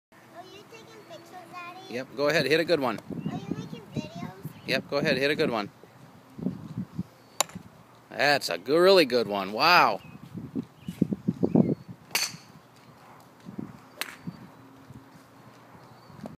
Ethan drives a golf ball July 24 2012
Almost 4 years old